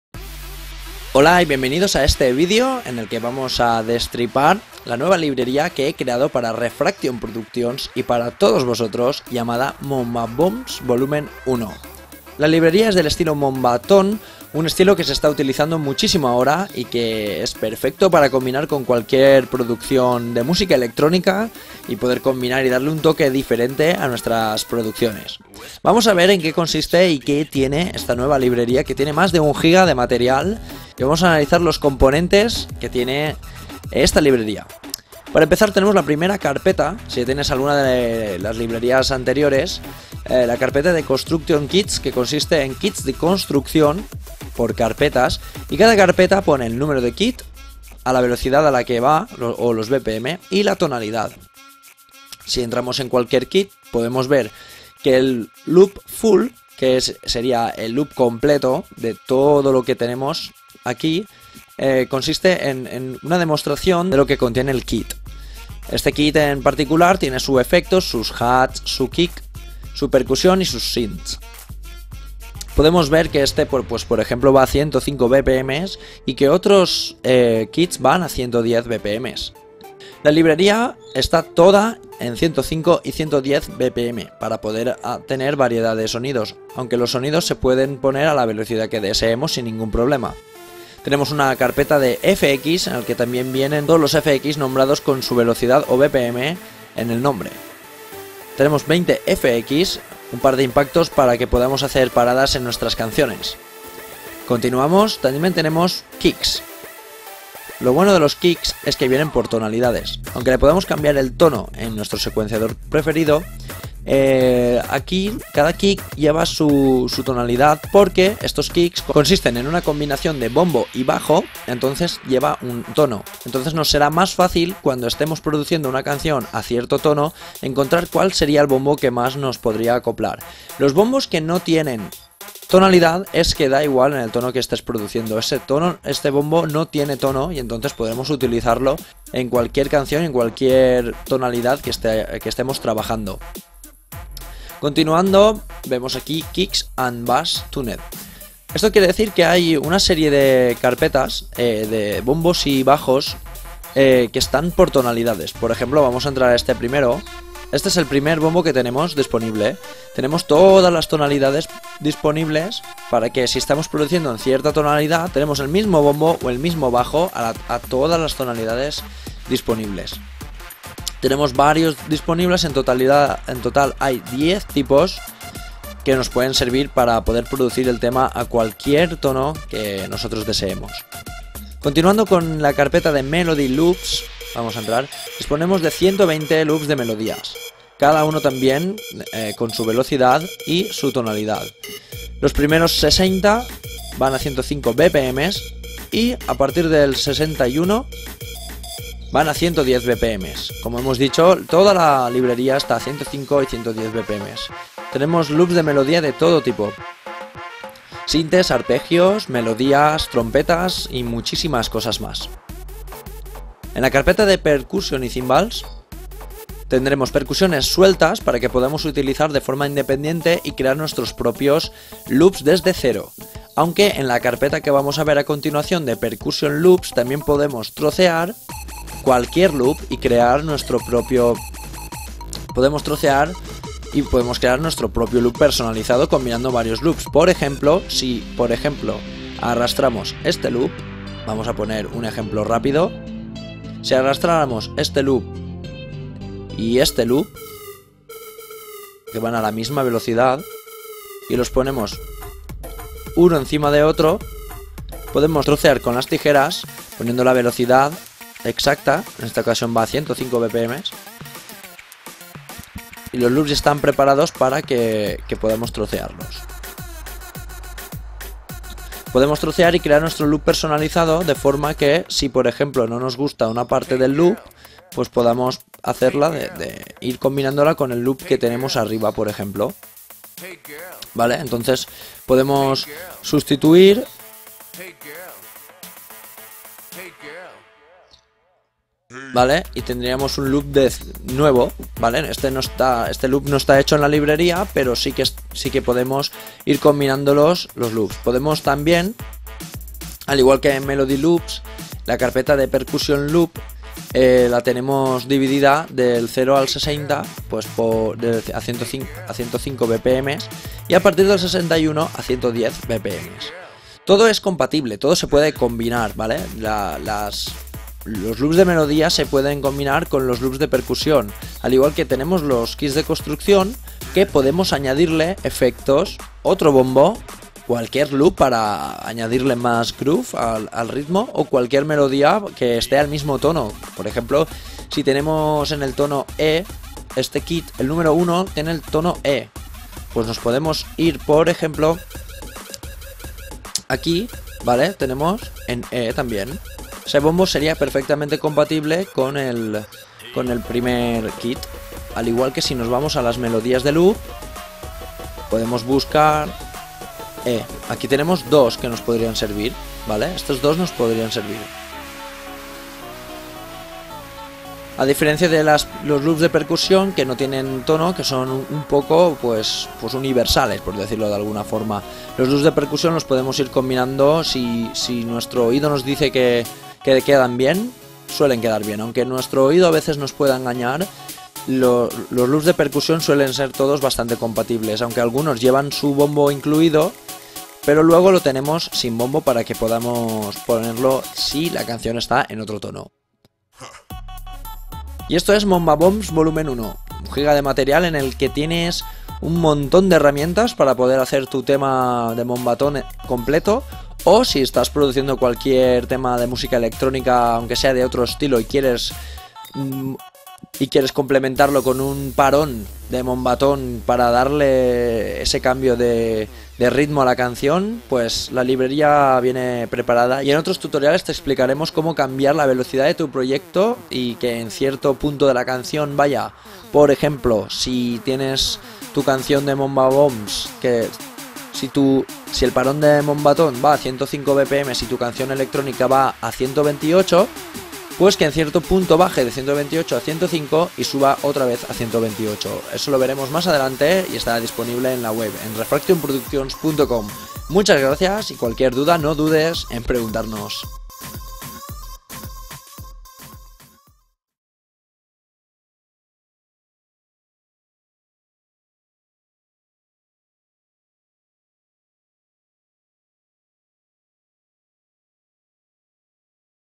Librería Moombahton - Loops y samples - Moombah Bombs Vol 1
-10 Kits de construcción por tonos incluyendo including percussions, bass, melodies/synths, fx, fills, ... (88 archivos)
-10 Instrumentos: escalas de Kicks y bajos (samples en A-G#) para Kontakt, Slicex (FL Studio) y en WAV samples
Analog Kick
Distort Bass
Dubstep Kick
Electro Bass
Hard Kick
Hip Hop Kick
Organ Bass
Saw Bass
Sine Bass
Trap Kick
-120 Melody tunes por tono
-20 FX samples
-40 Kicks por tono
-60 Percussion & Cymbal shoots/samples
-120 Percussion Loops

-Loops a 105 & 110 BPM
-Todos los archivos WAV Stereo 16bit, 44.1khz

En totalMás de 550 archivos en 1GB de material listo para importar a cualquier secuenciador o DAW

Vamos a a destripar la nueva librería que he creado para Refraction Productions y para todos nosotros llamada
Moombah Bombs Volumen 1, la librería del estilo moombahton
un estilo que se está utilizando muchísimo ahora y qué es perfecto para combinar con cualquier producción.

Si entramos en cualquier kit podemos ver que él loop Full qué sería el loop
completo de todo lo que tenemos consiste en una demostración de lo
que contiene el kit este kit en particular tiene su efecto sus hats, su kick su percusión y sus synths podemos ver que este pues por ejemplo
va a 105 bpm y que otros kits van a 110 bpm la librería está toda en 105 y 110 para
poder tener variedad de sonidos
aunque los sonidos se pueden poner a la velocidad que deseemos sin ningún problema.

Tenemos una carpeta de fx en el que también vienen todos los fx nombrados con su velocidad o bpm en el nombre tenemos 20 fx
un par de impactos para que podamos hacer paradas en nuestras canciones continuamos.

También tenemos kicks, lo bueno de los kicks es que vienen por
tonalidades aunque podamos cambiar el tono en nuestro secuenciador preferido aquí cada kick lleva su
tonalidad porque es que estos kicks consisten en una combinación del bombo y bajo entonces lleva en un tono entonces nos
será más fácil, sería el bombo que más nos podría acoplar, los bombos que no tienen tonalidad es que da igual el tono en que
estás produciendo, este bombo no tiene tono y entonces podemos utilizarlos en cualquier canción en cualquier
tonalidad que estemos trabajando.

Continuando vemos aquí kicks & bass tuned, que quiere decir que hay una serie de carpetas: bombos y bajos que están por tonalidades, por ejemplo vamos a entrar a este primero
este es el primer bombo que tenemos disponibles tenemos todas las
tonalidades disponibles para que si estamos produciendo en cierta tonalidad tenemos el mismo bombo o el mismo bajo a todas las tonalidades,
De melodías cada uno también tiene con su velocidad y su tonalidad los primeros 60 van a 105 bpm’s y a partir del 61 van a 110 bpm
como hemos dicho todas las librería
va entre 105 y 115 bpm tenemos loops fe melodía de todo tipo sintetizadores, arpegios, melodías trompetas y muchísimas cosas más

En la carpeta de percusión y cymbals tendremos percusiones sueltas para que podamos utilizar de forma independiente y crear nuestros propios loops desde cero
aunque en la carpeta que vamos a ver a continuación de percusión loops también
podemos trocear cualquier grupo y crear nuestro propio
podemos trocear y podemos crear nuestro propio loop personalizado combinando varios loops
La tenemos dividida del 0 al 60 pues a 105, a 105 bpm y a partir de 61 a 110 bpm
todo es compatible todo se puede combinar, vale?

Los loops de melodías se pueden combinar
con los loops de percusión al igual que tenemos los kits de
construcción que podemos añadirle efectos, otro bombo,
cualquier club para añadirle más groove al ritmo o cualquier melodía que esté al mismo tono por ejemplo si
tenemos en el tono E, este kit el número uno en el tono E pues podemos y por ejemplo aquí, tenemos E también
este bombo sería perfectamente compatibles con él con el primer kit al igual que si nos vamos a las
melodías de loops podemos buscar aquí tenemos dos que nos pueden servir
con estos dos nos podrían servir a diferencia de los loos de percusión

Y esto es moombah bombs volumen 1, 1 giga de material en el que tienes un montón de herramientas para poder hacer tú tema de moombahton completo o si estás produciendo cualquier tema de música electrónica aunque sea de otro estilo y quieres y
quieres complementarlo con un parón de moombahton  para darle ese cambio de ritmo a la canción pues la librería viene preparada.

En otros tutoriales te explicaremos cómo cambia la velocidad de tu proyecto y que en cierto punto de la
canción vaya por ejemplo sí tienes tu canción de moombahton  si el parón de moombahton va
a 105 bpm y tú canción electrónica va a a 128 pues que en cierto punto base de 128 a 105
y suba otra vez a 128 eso lo veremos más adelante